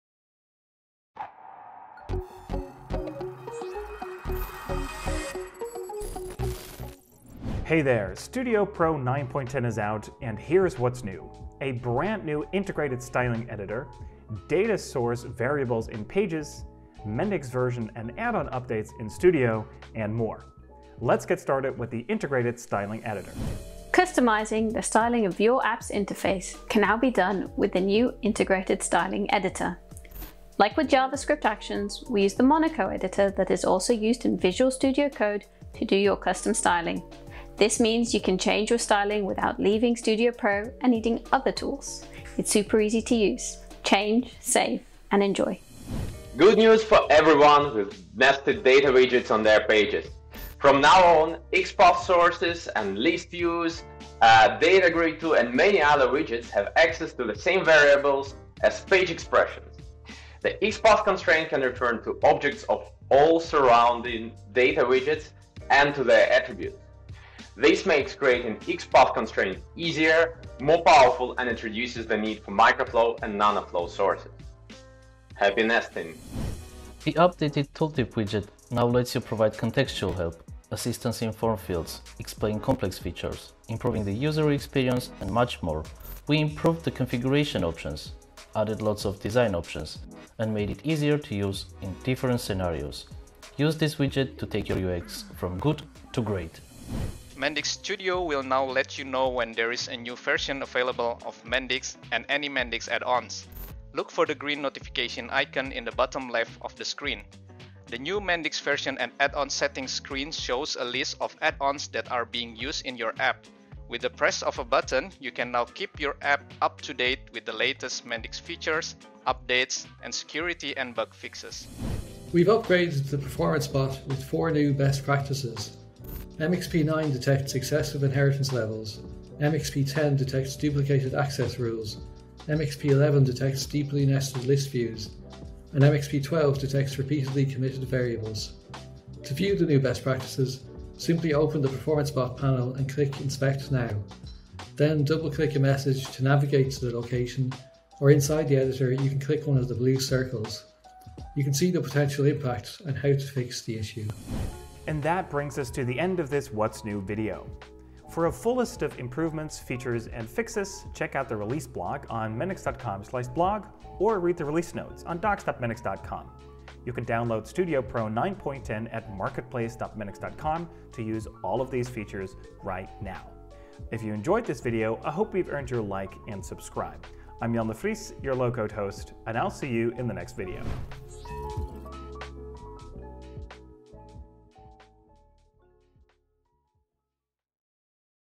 What's New: Styling Editor, Tooltip Improvements, Performance Bot - Mendix 9.10
This video covers:
- Integrated styling editor
- Data Source Variables in Pages
- Tooltip widget update
- Studio: Mendix version and add-on module updates
- New best practice for MxAssist Performance Bot